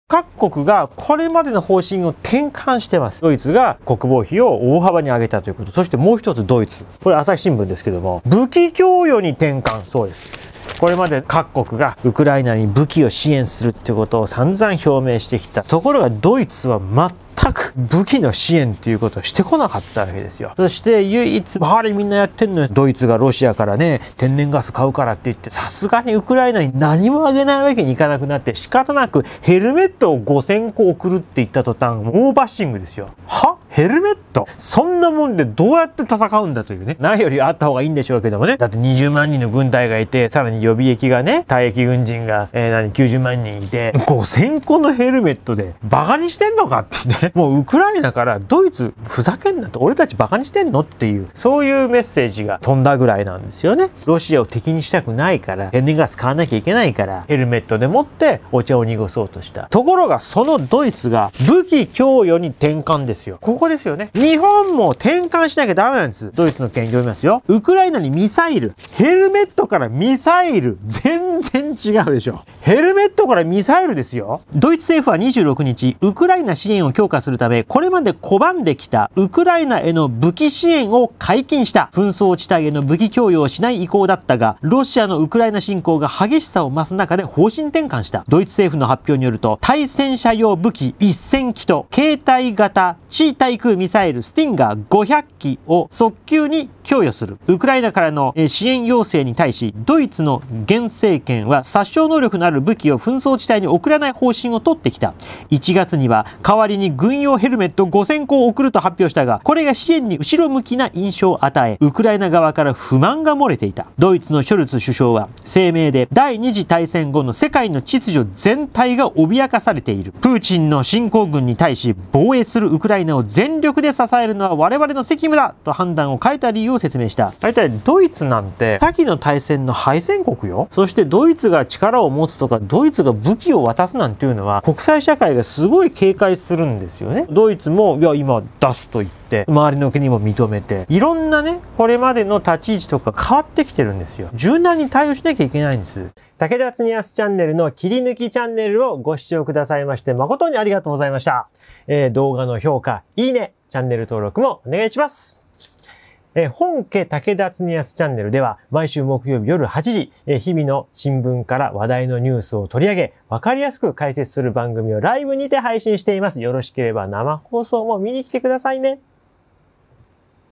ロシアやべー！さすがのドイツもヘルメットから武器支援にウクライナ支援方針を転換
あのドイツが！ヘルメットを送っていたドイツが
ウクライナに武器支援をする事に！
ドイツをここまで踏み切らせたロシアの行動がどれだけヤバいか！